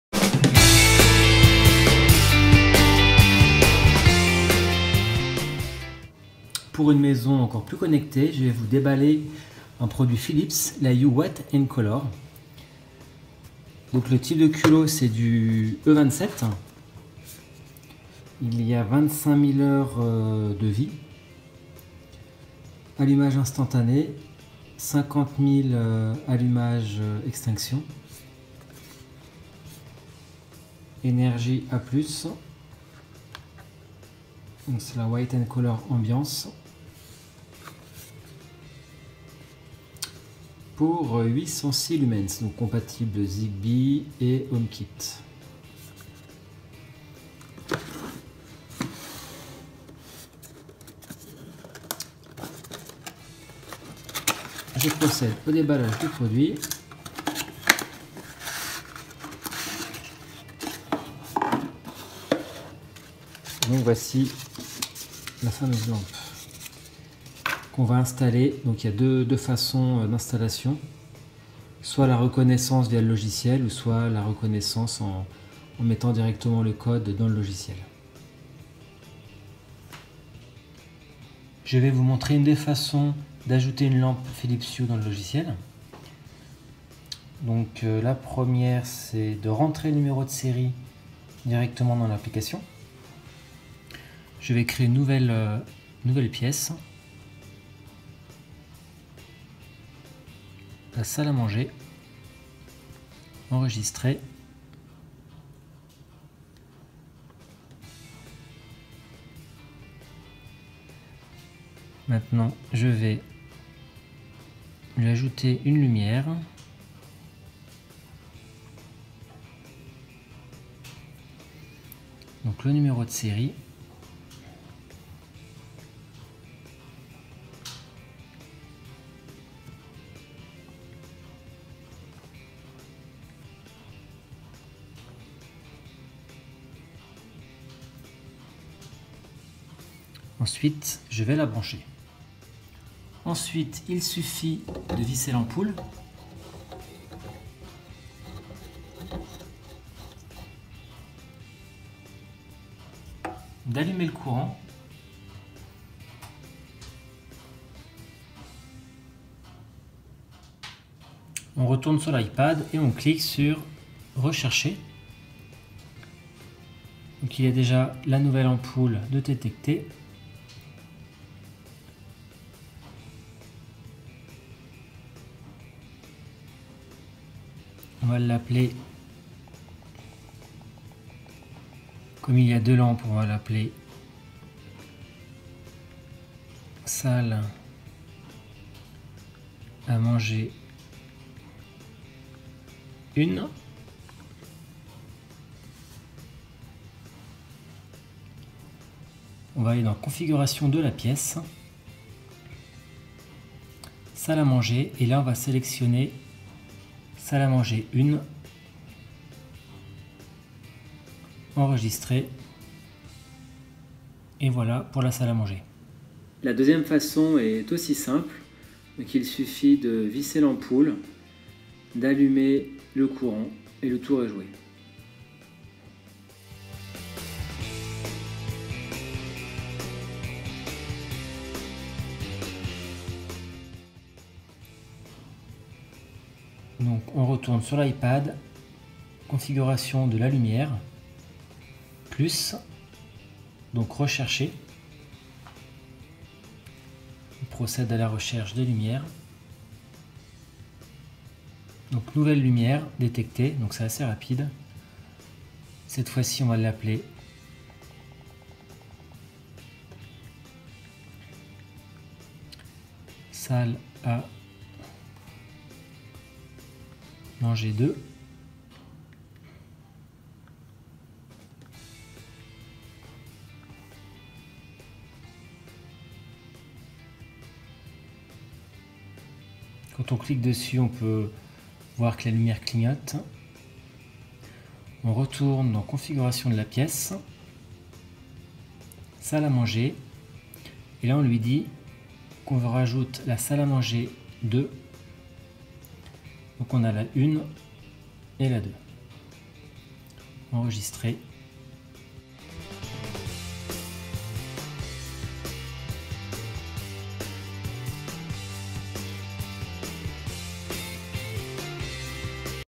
Unboxing / déballage ampoule Philips Hue white and color et installation compatible HomeKit
Ampoule Philips Hue white and color ambiance
Ampoule standard culot à vis - E27
Luminaire à base filetée 10 W
Durée de vie de 25 000 heures
Tension d’entrée : 220 V-240 V
Flux lumineux de l'ampoule Philips Hue white and color ambiance
806 lm @ 4 000K
Lumière blanche fonctionnelle
16 millions de couleurs
Toutes les nuances de blanc, des températures de couleur chaudes à froides
Largeur de faisceau –160 ° ± 20 °

BTC: 1P5c9d7Di7waBKZ8RjvQYWht5T7nAP3dMn
BCH: pptfjwuxwxwzd7z56a2fhmund55z5gas9uzpx9wpnq